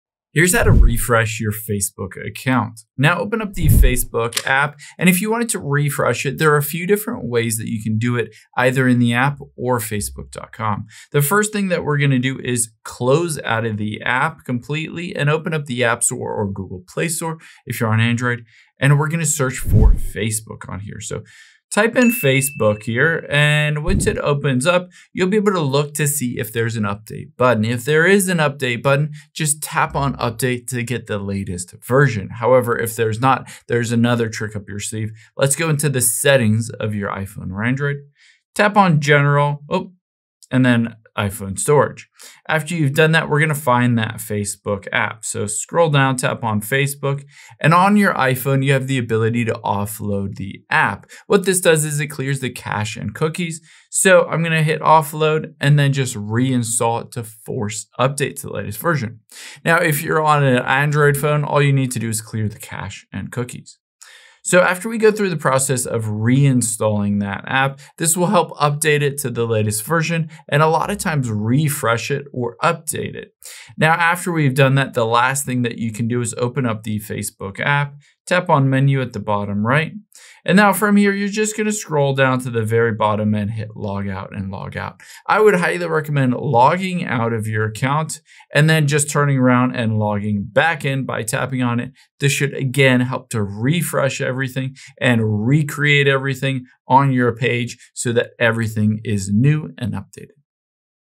How To Refresh Facebook Account
Here's how to refresh your Facebook account if it has bugs, you want the latest features, etc.